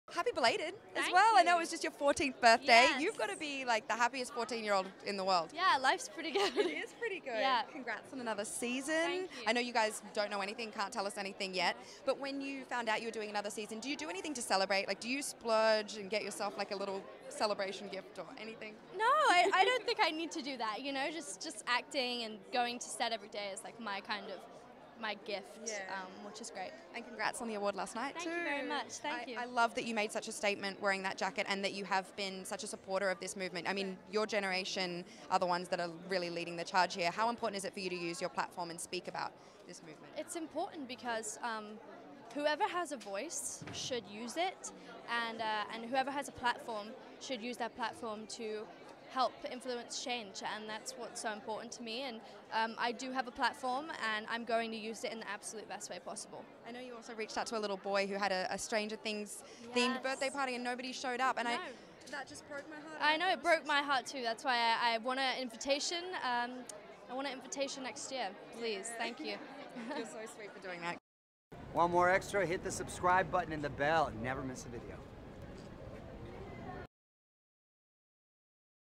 Millie Bobby Brown Talks ‘Stranger Things’ Season 3 and More
“Extra’s” Renee Bargh caught up with Millie Bobby Brown at PaleyFest 2018 where she talked about “Stranger Things” Season 3, the MarchForOurLives movement, and more.